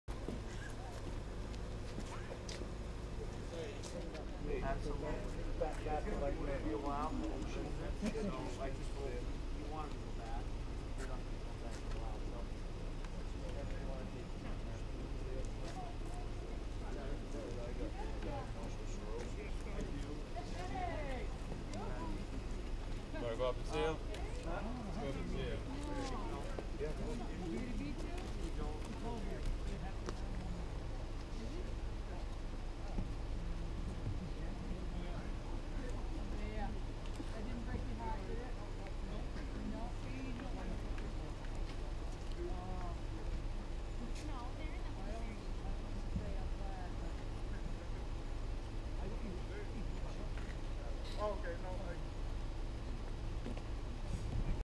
Livermore Falls, Maine family reunites after hostage situation
Livermore Falls, Maine family reunites after day long ordeal being held hostage by a lone person. Officials on scene say all hostages are now "safe" STORY